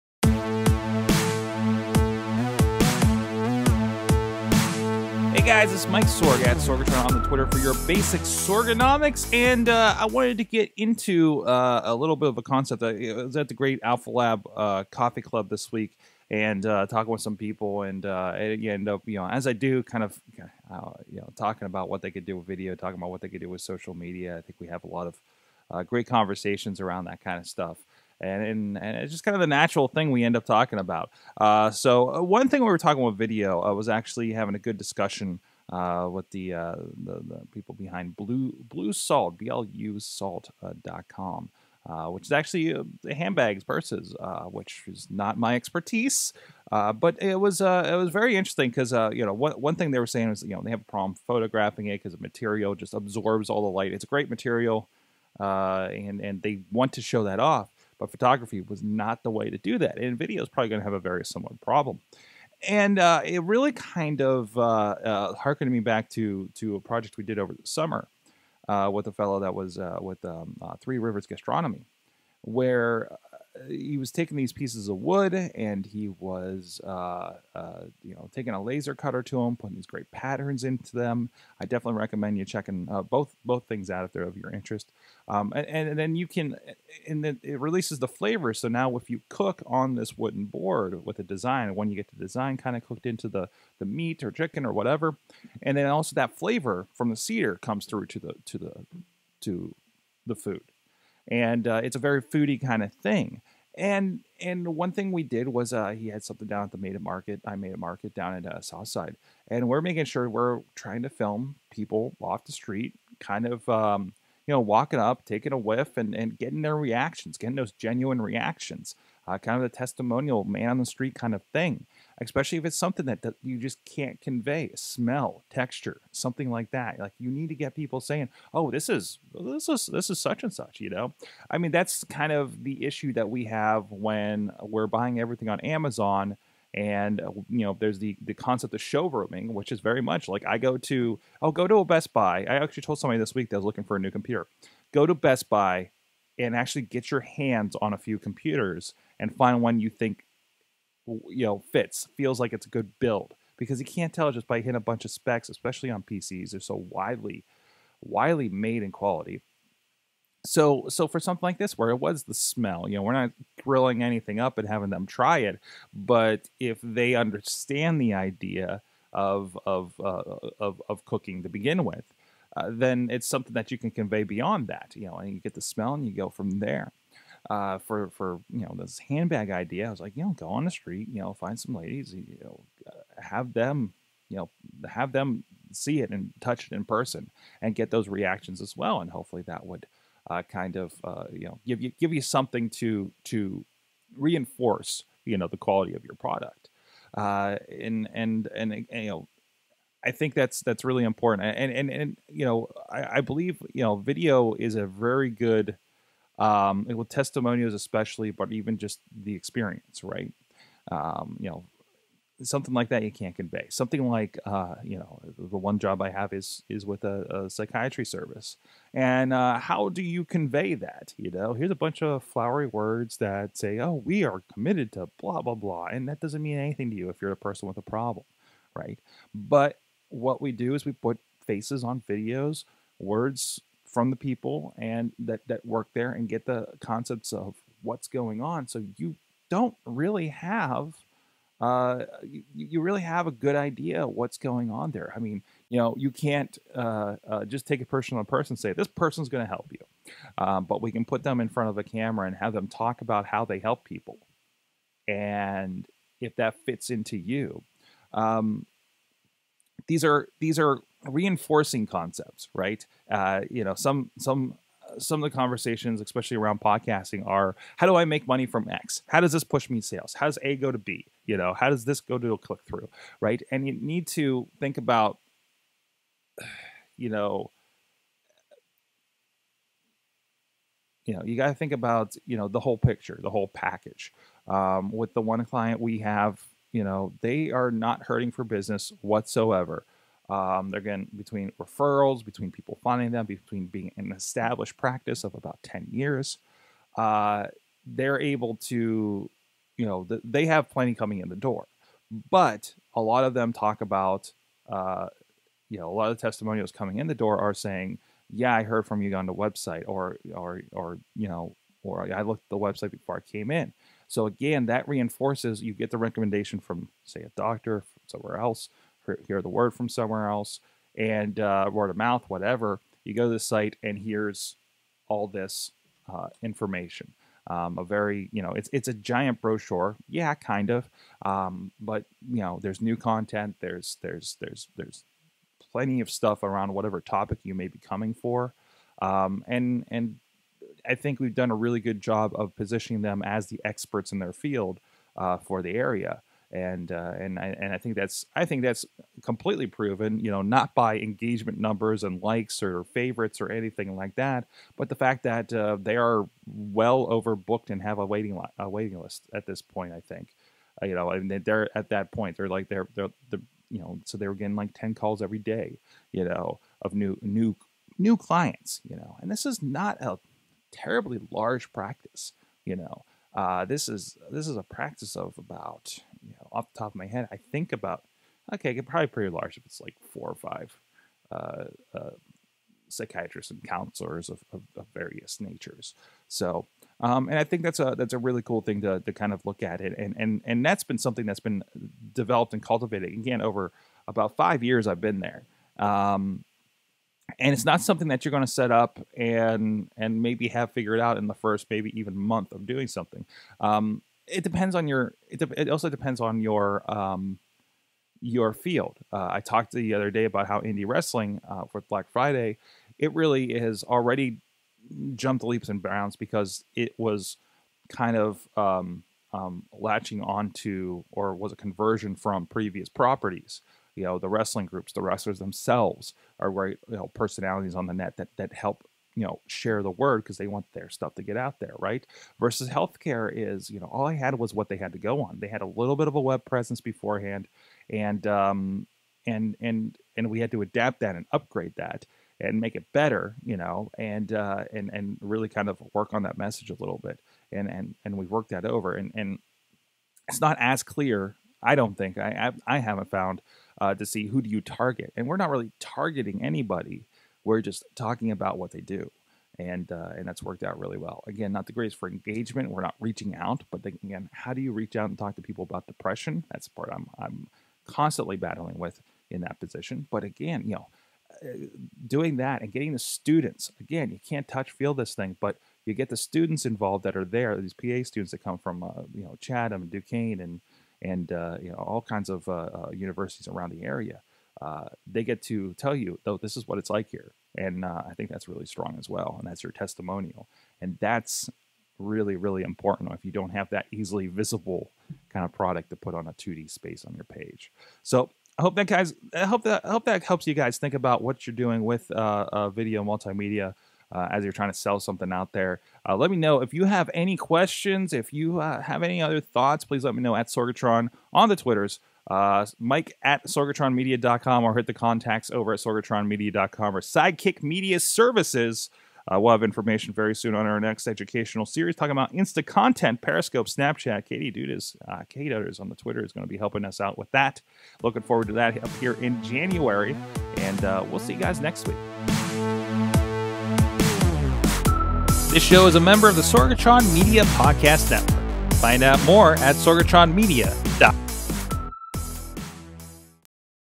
Basic Sorganomics: Auxiliary Content
Today, we talk about using video to convey products that require smell or touch to experience them, and using a web site not to generate click throughs, but to create an information resource for a practice to help ease new clients in the door.